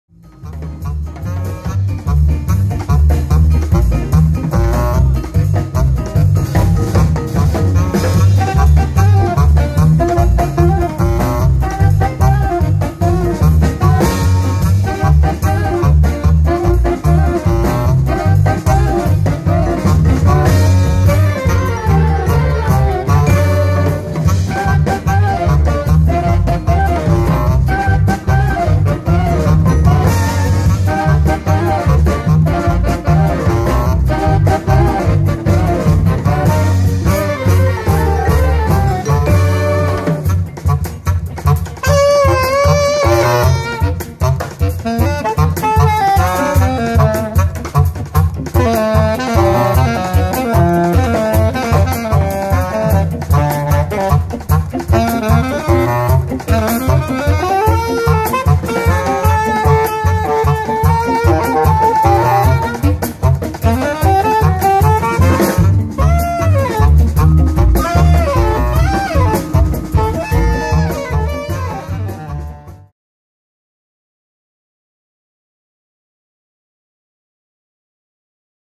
Südtirol Jazzfestival Alto Adige 2017: Jazz & Herbs feat. Euregio Jazzwerkstatt - 3 July 2017
Euregio Jazzwerkstatt @ Pflegerhof / Seis (South Tyrol)